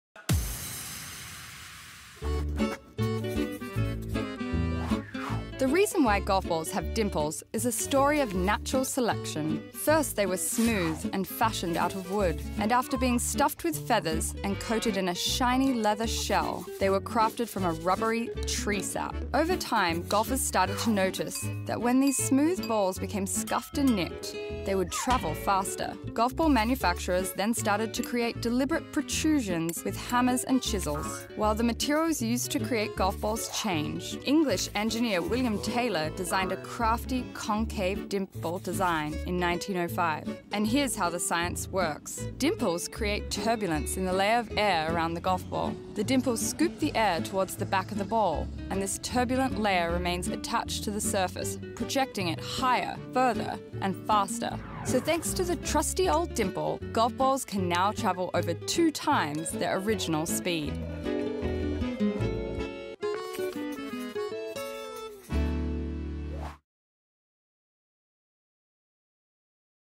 GOLF SCIENCE - WHY GOLF BALLS HAVE DIMPLES
Do the dimples make the ball go faster? Do the dimples make the ball go further? We find out in this weeks golf science.